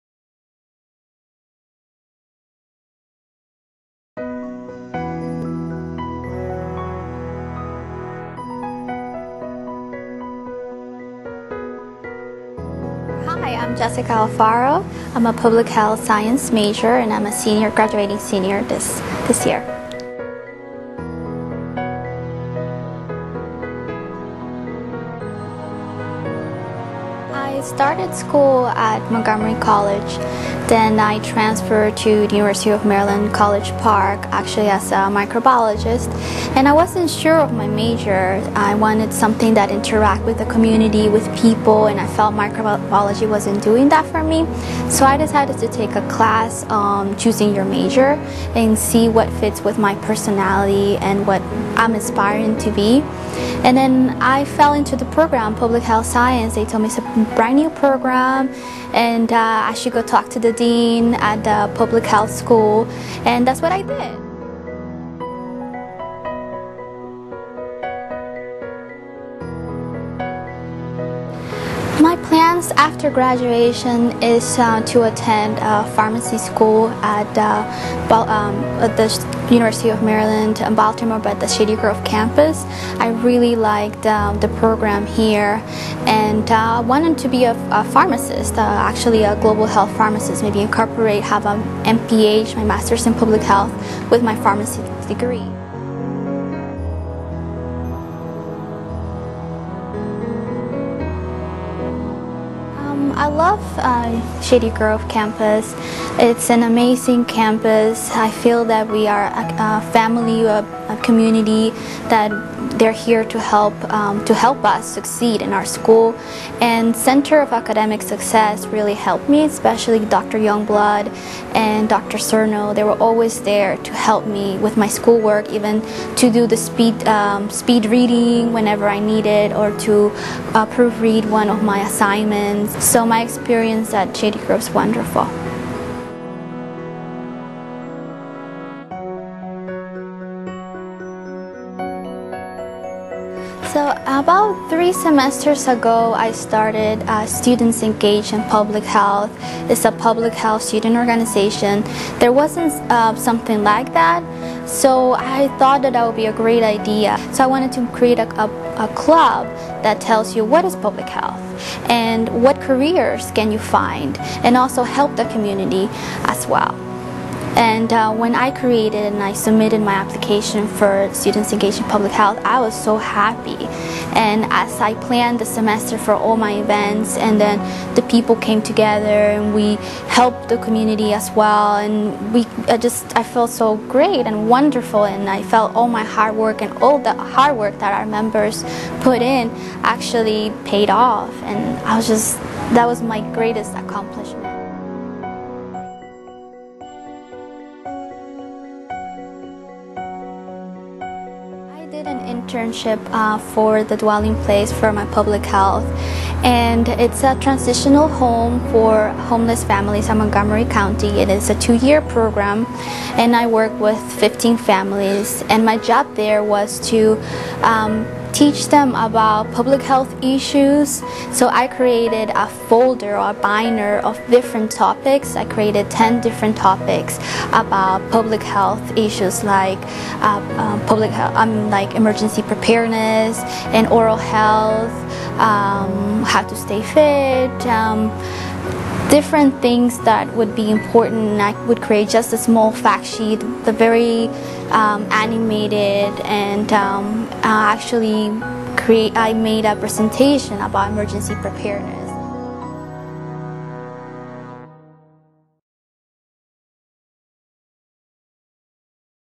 Student Interview: Jessika Alfaro, Public Health Science
Graduating senior, Jessika Alfaro, discusses her experience in the UMCP